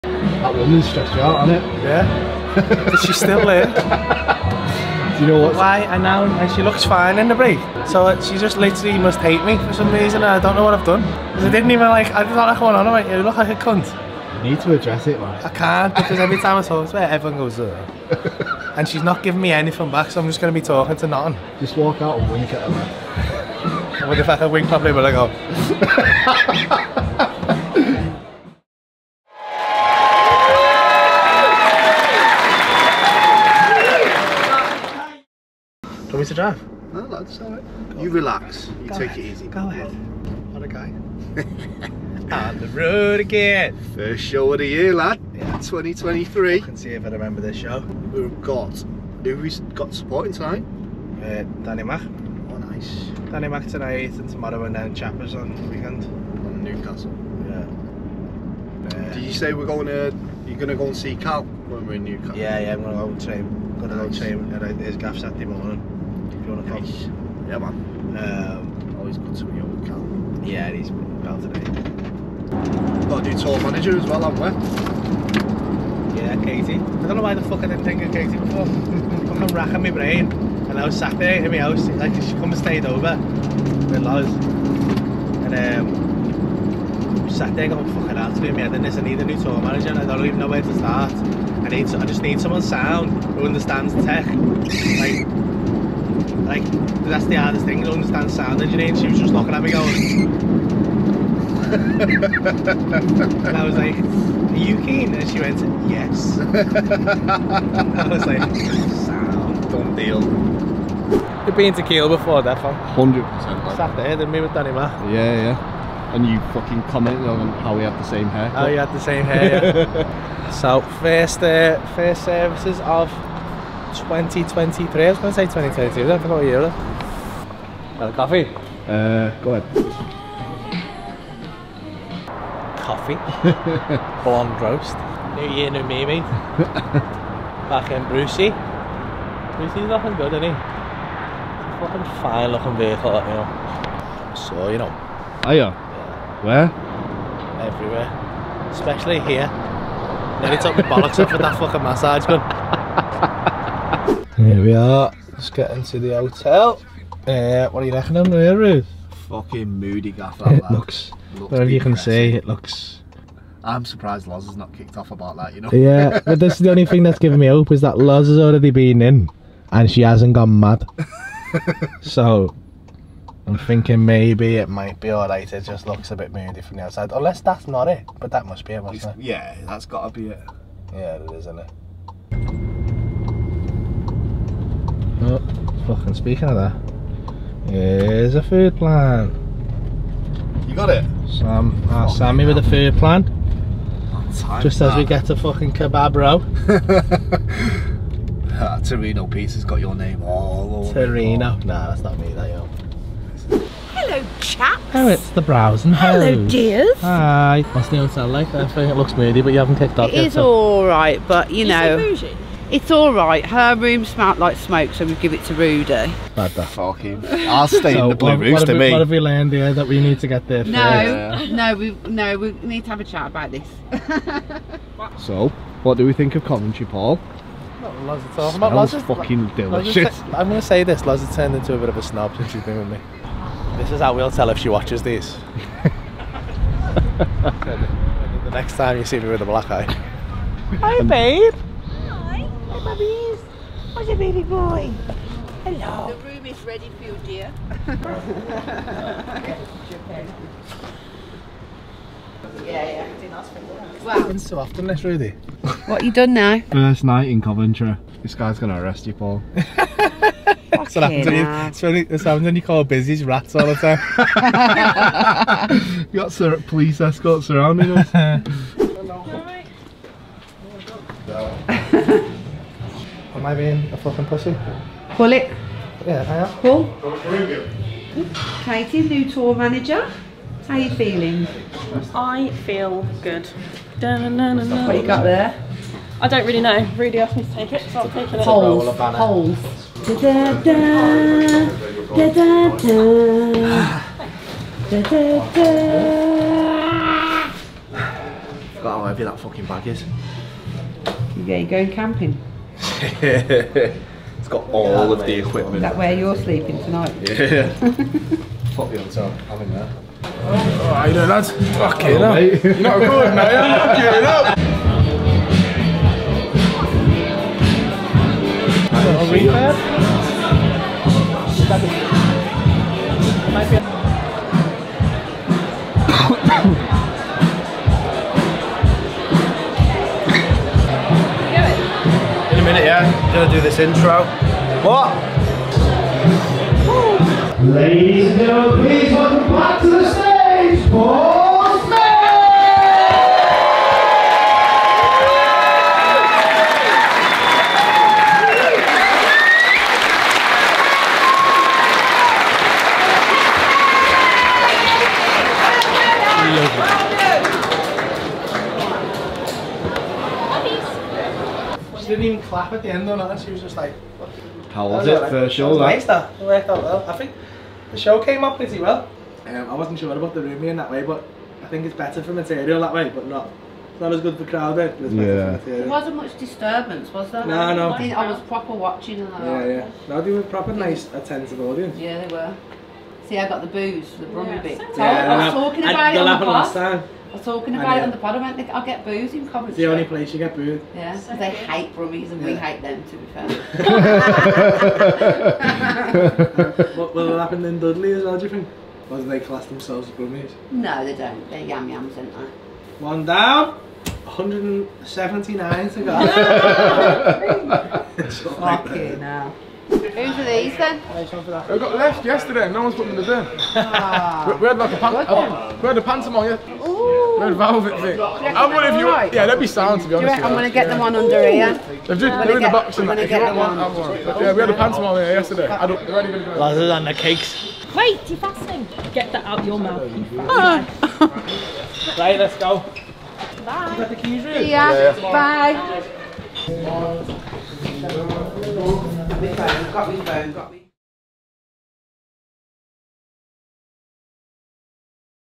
Paul Smith | Joker 2023 Tour | Behind The Scenes | Coventry Part One
We kick off 2023 with the first tour show of the year at the Rialto Plaza, we have a new tour manager Katie and we speak with a woman who clearly doesn't want to see Paul on the front row!.

Directed & Produced by Rudie Chiu - RDC Creative Media